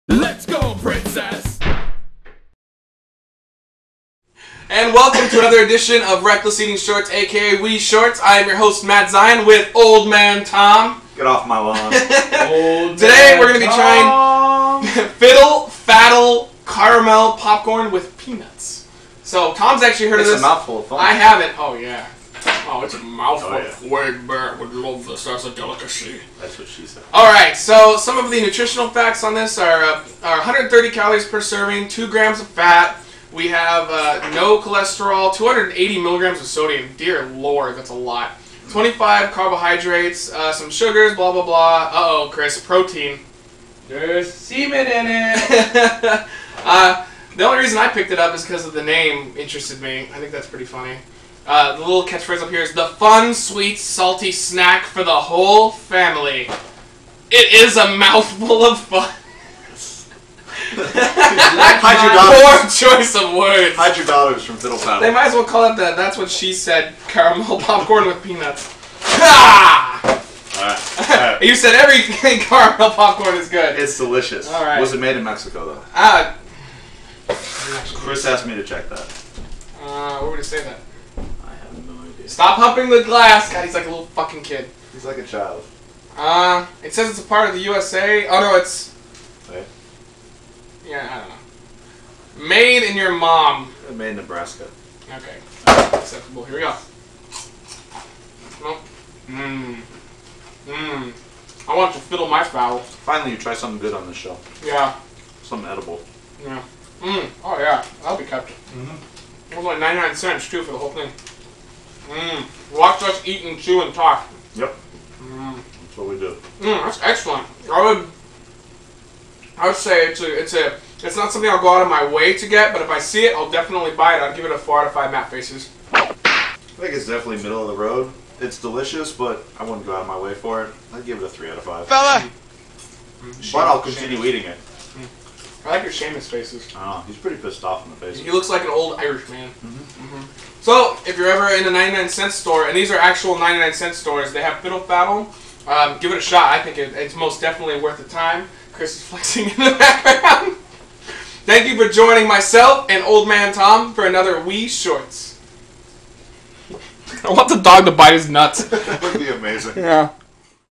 WE Shorts - Fiddle Faddle Caramel
Today on we shorts Zion & Tom take a look at Fiddle Faddle Caramel!